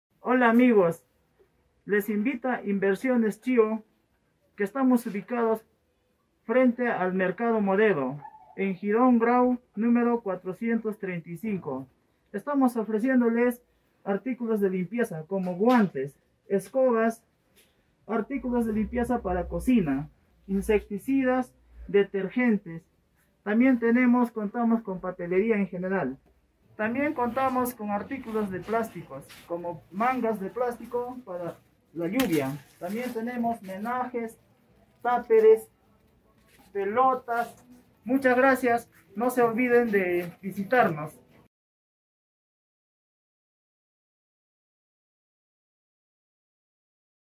Somos Chamba - Guido Quispe Huayhua - Junín
Guido Quispe es propietario de “Inversiones Chio” una empresa que se dedica a la venta de artículos de limpieza como escobas, detergentes, limpiadores, insecticidas, papel higiénico, etc. También cuentan con mangas de plástico, menaje en general, utensilios, tapers, pelotas y mucho más.

Jr. Miguel Grau 435 Chupaca, Chupaca - Junín